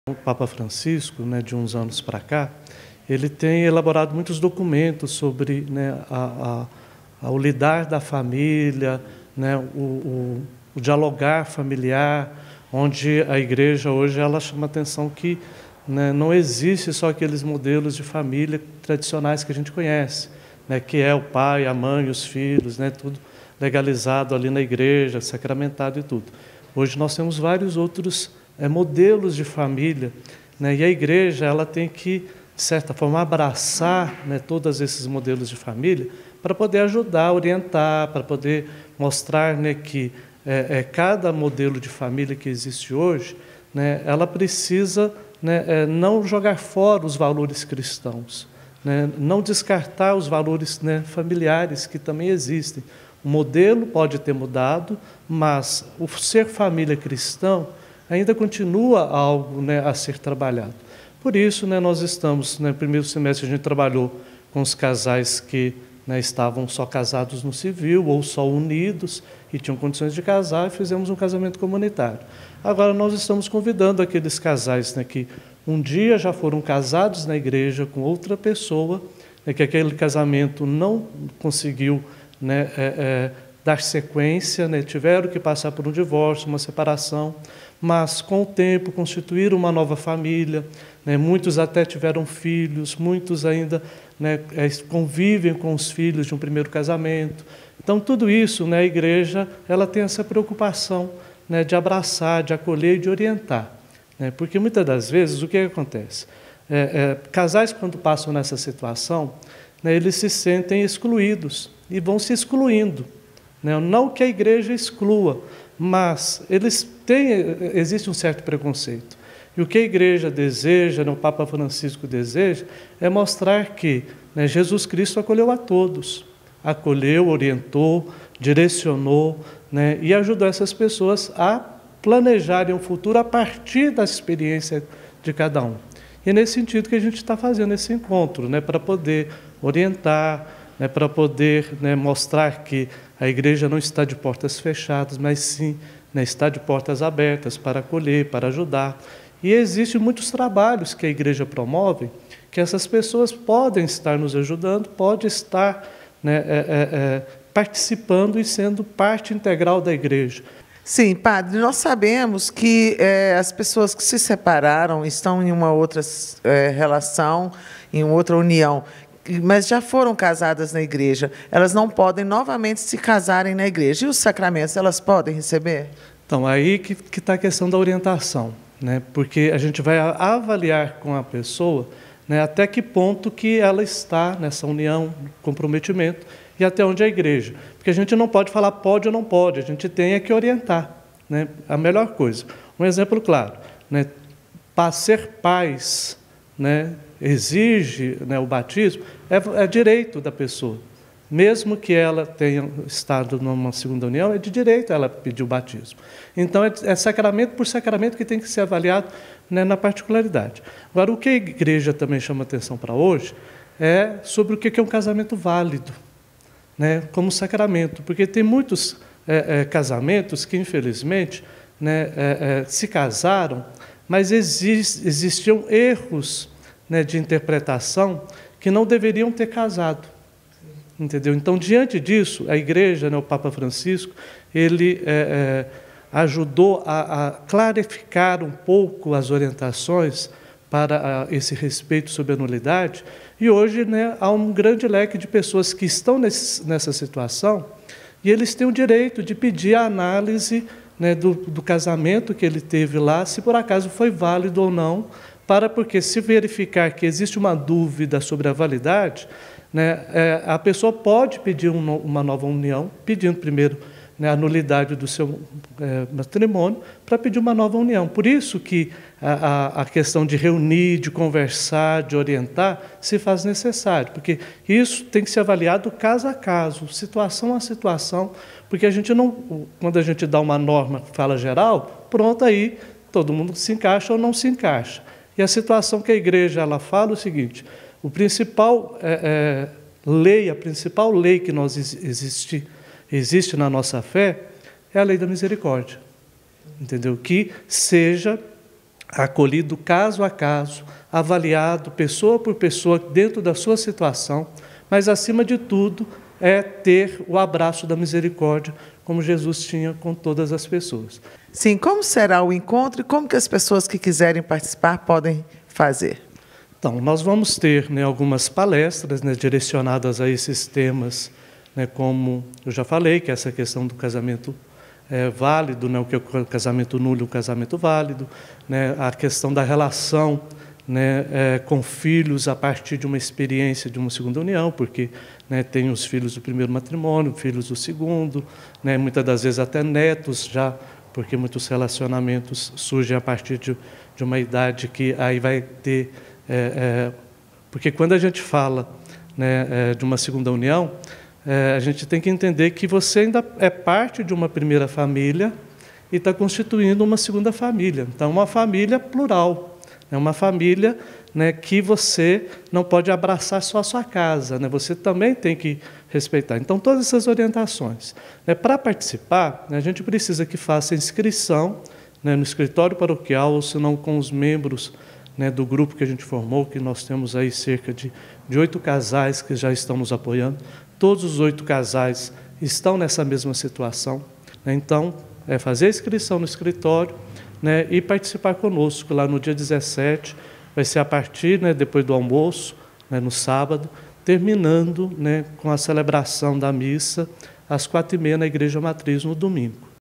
PARÓQUIA NOSSA SENHORA DO LIVRAMENTO DE PIUMHI PROMOVE O 1º ENCONTRO DE CASAIS EM SEGUNDA UNIÃO
| PIUMHI |